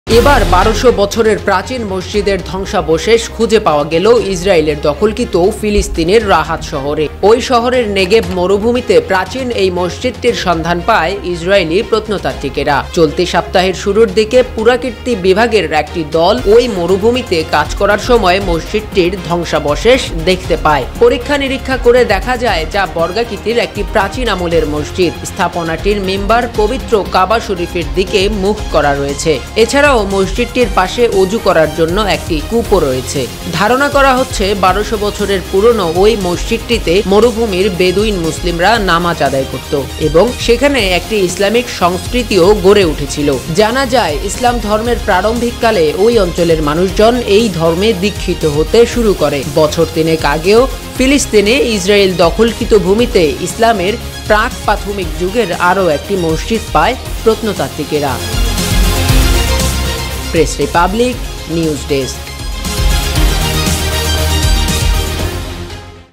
ইসরায়েলে ১২০০ বছরের প্রাচীন মসজিদের সন্ধান || PRESS Republic
এবার ১২০০ বছরের প্রাচীন মসজিদের ধ্বংসাবশেষ খুঁজে পাওয়া গেল ইসরায়েলের দখলকৃত ফিলিস্তিনের রাহাত শহরে।

মাত্র ১৪ বছর বয়সে বিয়ে করে হলেন সন্তানে বাবা || Saudi Arab | NEWSMAN

NewsMAN is a Multimedia News Portal.